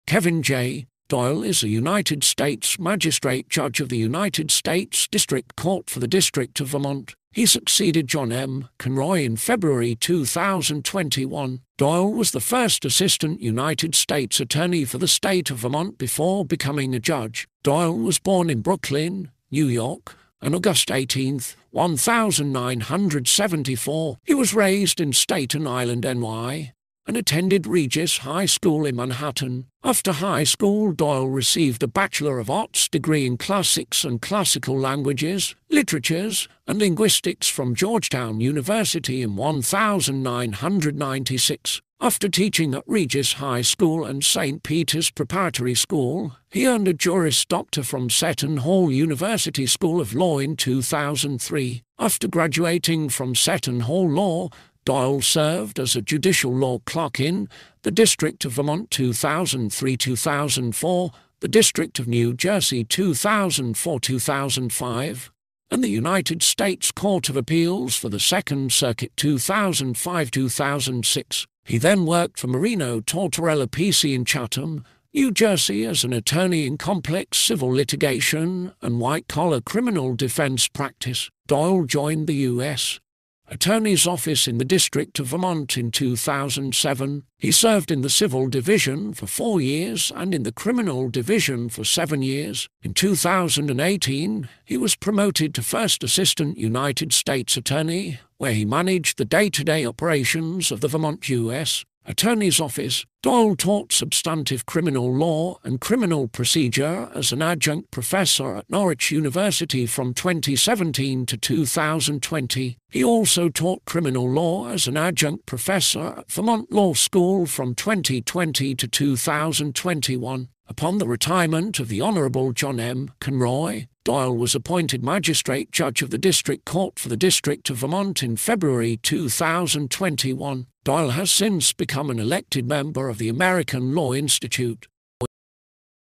Kevin J. Doyle Biography
This video is about Kevin J. Doyle Biography in English. Kevin J. Doyle is a United States Magistrate Judge of the United States District Court for the District of Vermont. He succeeded John M. Conroy in February 2021. Doyle was the first Assistant United States Attorney for the state of Vermont before becoming a judge.
Doyle was born in Brooklyn, New York, on August 18th, 1974. He was raised in Staten Island NY, and attended Regis High School in Manhattan. After high school Doyle received a Bachelor of Arts degree in classics and classical languages, literatures, and linguistics from Georgetown University in 1996. After teaching at Regis High School and St. Peter's Preparatory School, he earned a Juris Doctor from Seton Hall University School of Law in 2003.
After graduating from Seton Hall Law, Doyle served as a Judicial Law Clerk in: The District of Vermont (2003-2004), The District of New Jersey (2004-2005), and The United States Court of Appeals for the Second Circuit (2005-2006). He then worked for Marino Tortorella PC in Chatham, New Jersey as an attorney in complex civil litigation and white collar criminal defense practice.
Doyle joined the U.S. Attorney’s Office in the District of Vermont in 2007. He served in the Civil Division for four years and in the Criminal Division for seven years. In 2018, he was promoted to First Assistant United States Attorney, where he managed the day-to-day operations of the Vermont U.S. Attorney’s Office. Doyle taught substantive criminal law and criminal procedure as an adjunct professor at Norwich University from 2017 to 2020. He also taught criminal law as an adjunct professor at Vermont Law School from 2020 to 2021.
Upon the retirement of The Honorable John M. Conroy, Doyle was appointed magistrate judge of The District Court for the District of Vermont in February 2021. Doyle has since become an elected member of the American Law Institute.